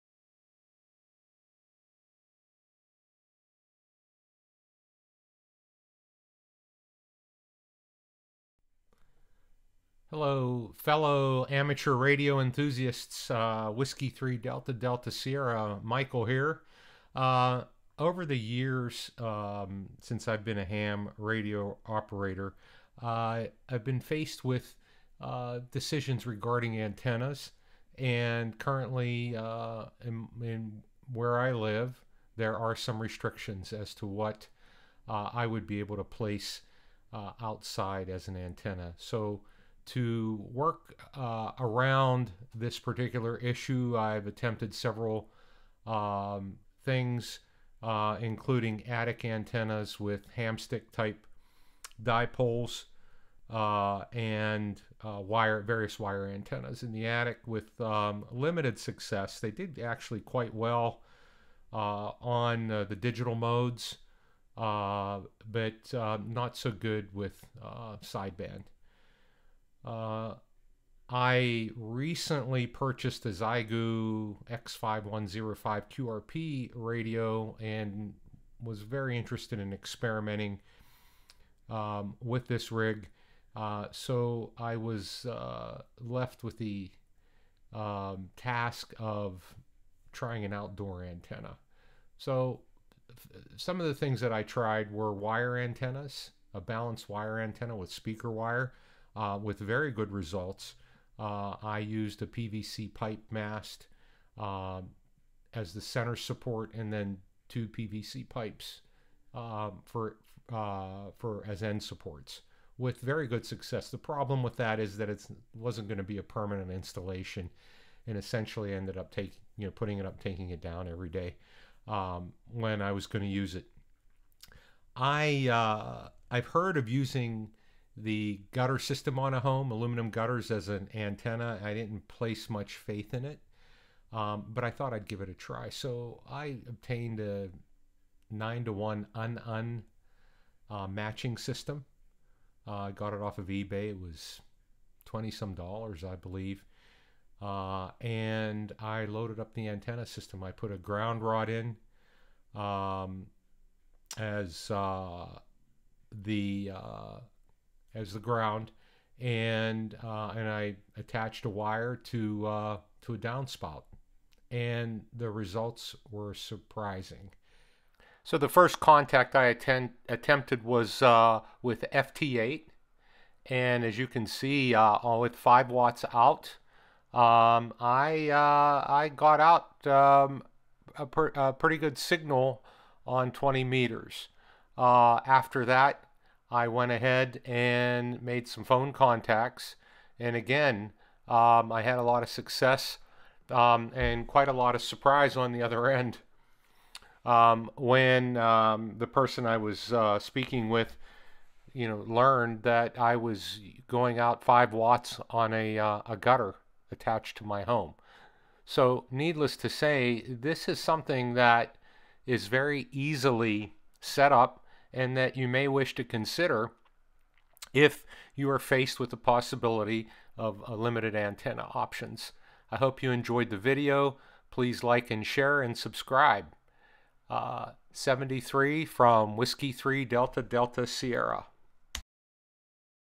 HF Gutter Antenna for Amateur Radio
For those with limited antenna options this is something to consider.  The results were surprising to say the least. Contacts were made with my XIEGU X5105 QRP 5 watts using FT8 and SSB.  Please keep in mind that an antenna tuner is required.